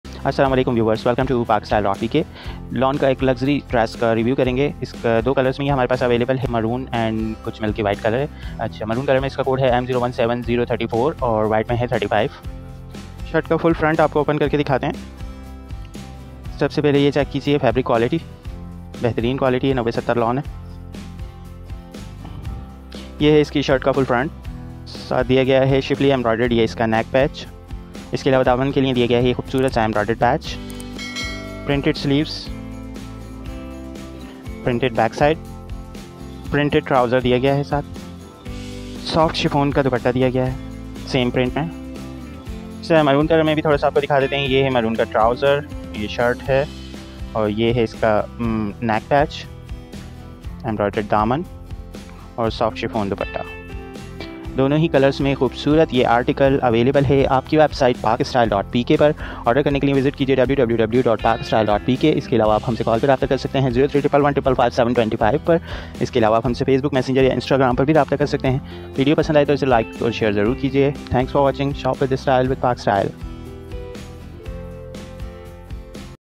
Designer Pakistani Lawn Dresses Design 2024 | Fancy Summer Suits Online Shopping in Pakistan
Fancy Pakistani Lawn Suits Designs Review | Women's Lawn Dresses 2024 | Lawn Dresses Online Shopping

For Order:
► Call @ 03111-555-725